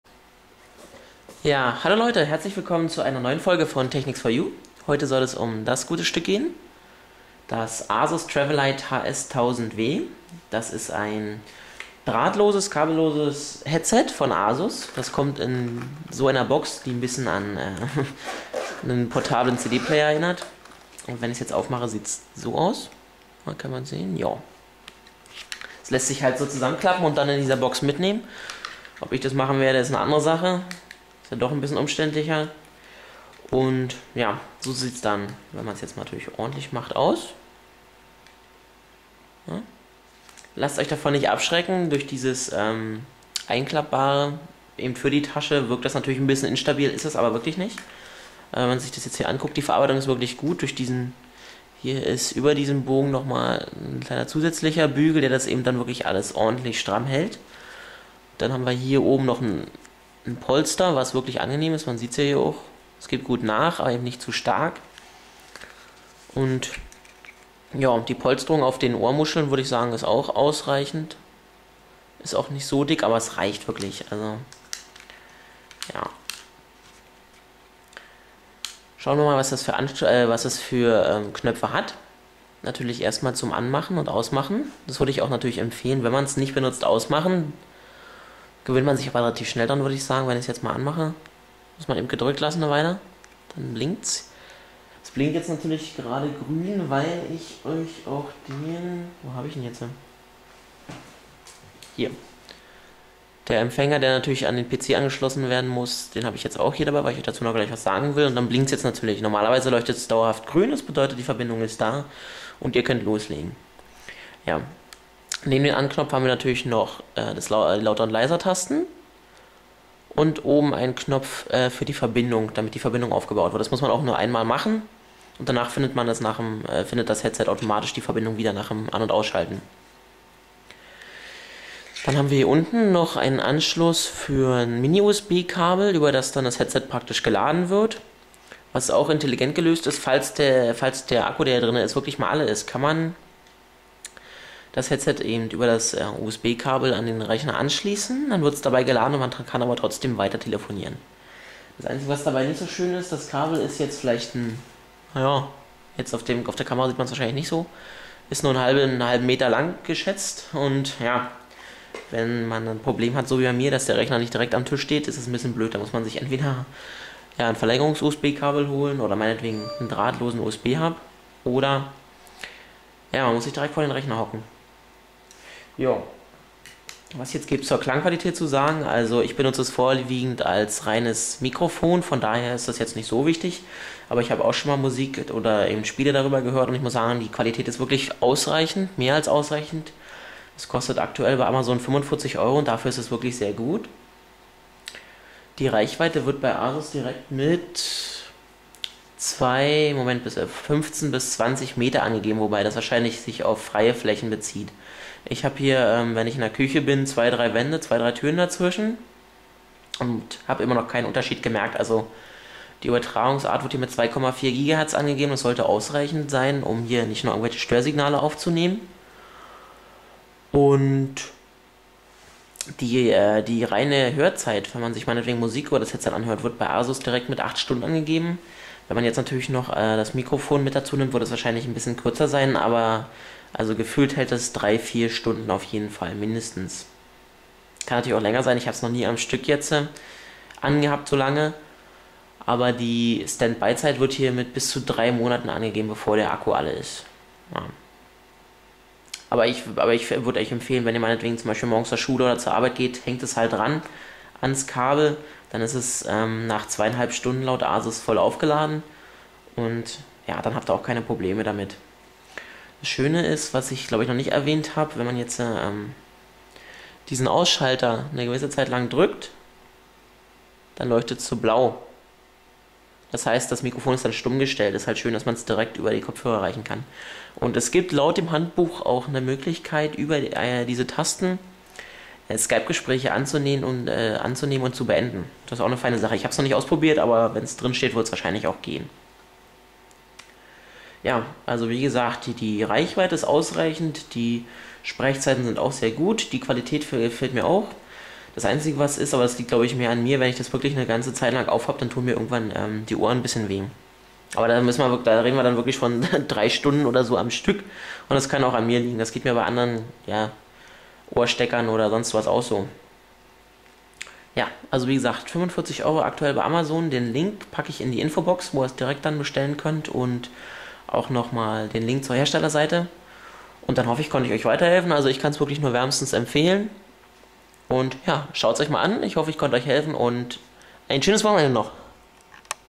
[Review] Asus TRAVELITE HS-1000W - Kabelloses Headset
Ja, ich habe ein kabelloses/wireless Headset gesucht, dass nach Möglichkeit eine große Reichweite und lange Gesprächszeit hat. Zuerst wollte ich mich für eine teurere Variante von Logitech entscheiden, hab dann dem Asus TRAVELITE HS-1000W doch eine Chance gegeben und ich bin voll zufrieden damit.

TRAVELITE HS-1000W an die Playstation 3 anschließen: